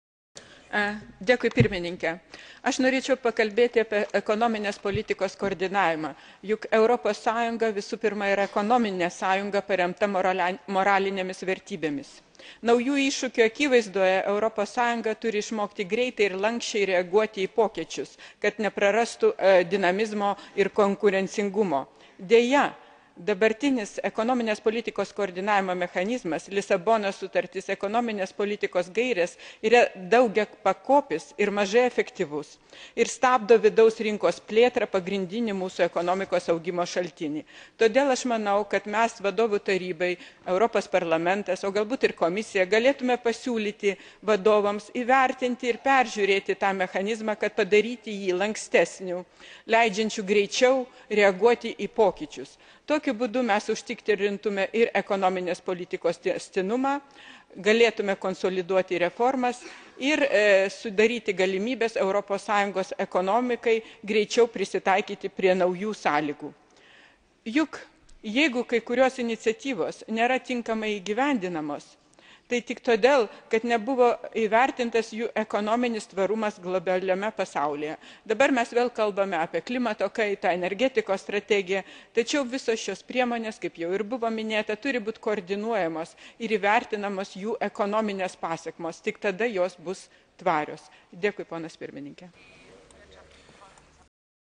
Margarita Starkevičiūtė on Preparation of European Council
Speech by Margarita Starkevičiūtė MEP (ALDE-ADLE) on :

Preparation of the European Council (Brussels, 13-14 March 2008)

[MEP Speech] [Language LT original]

Starkevičiūtė_080312_104900_mesp_lt